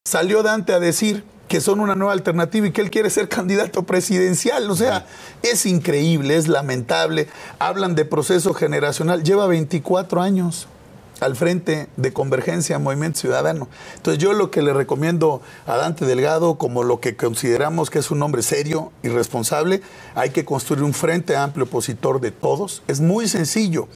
Alejandro Moreno, presidente del PRI
“Movimiento Ciudadano habla de que son una nueva alternativa y proceso generacional pero Dante Delgado lleva 24 años al frente de Convergencia - MC”, Alejandro Moreno, presidente del PRI